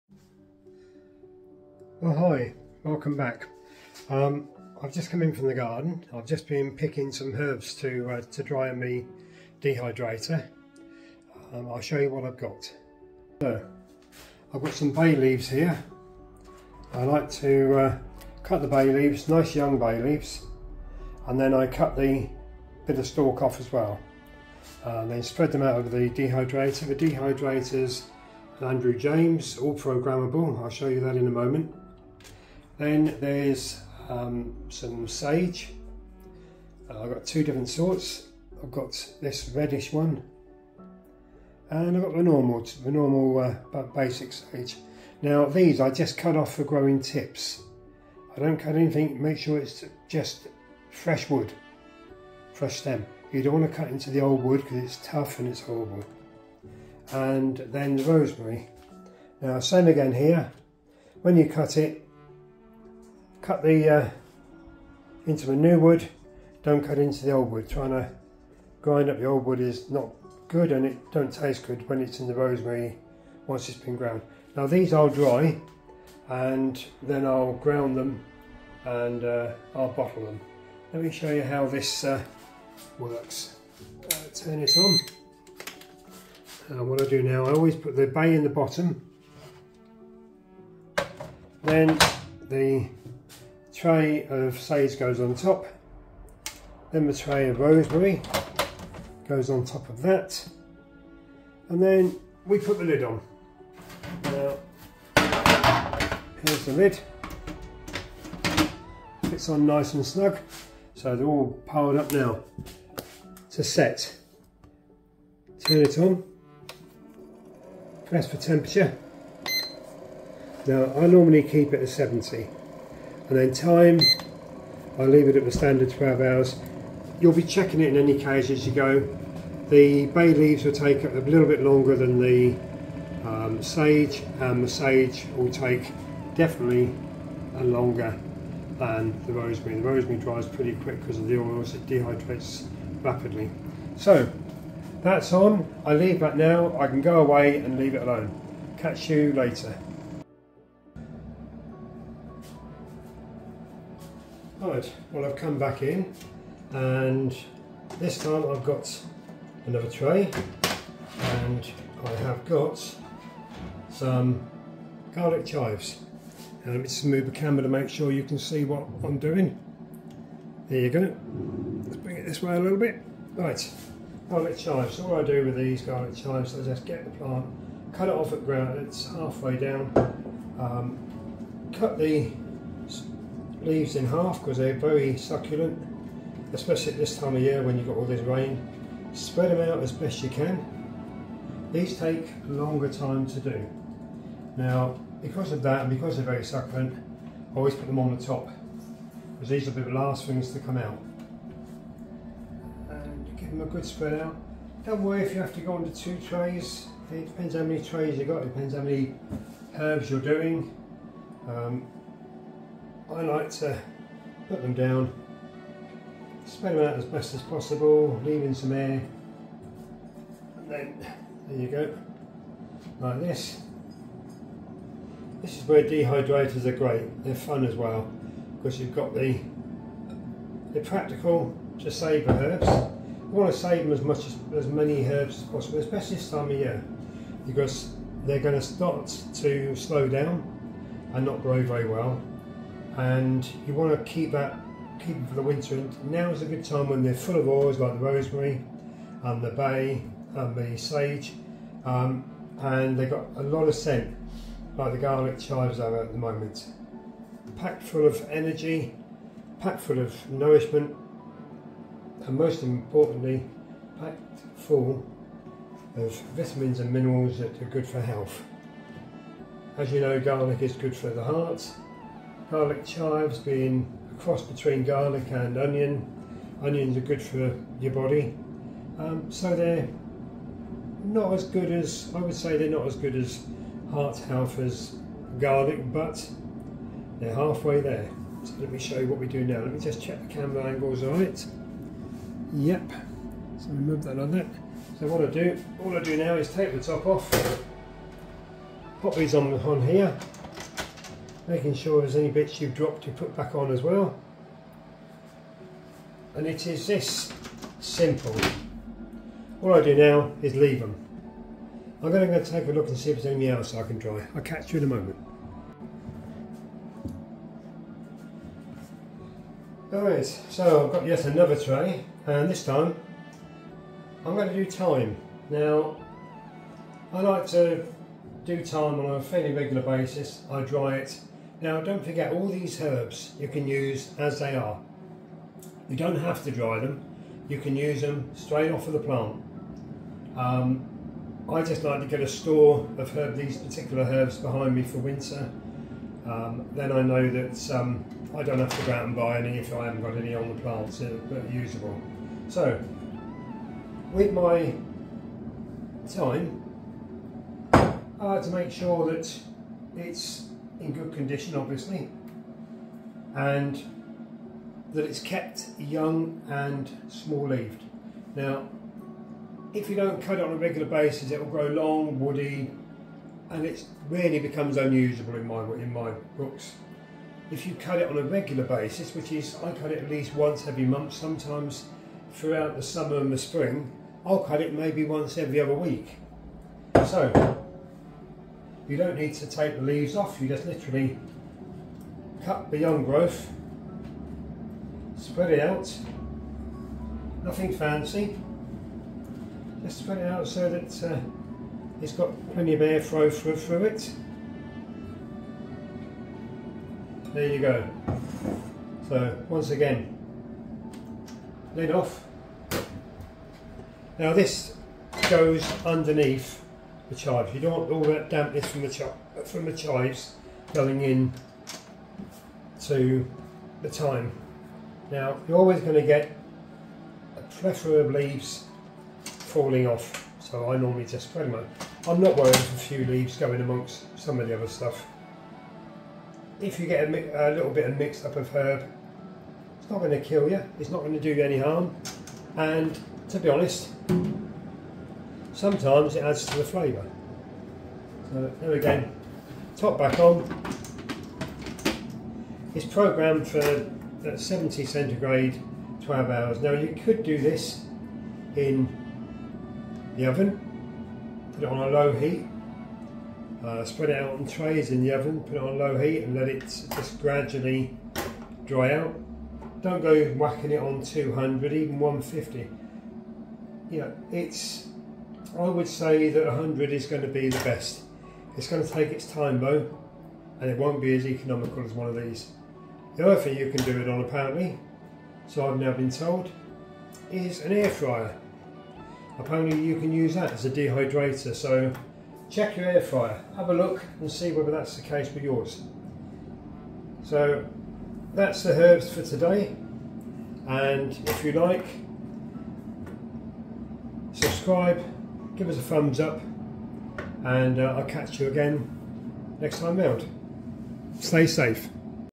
in the kitchen
I'm drying herbs for winter.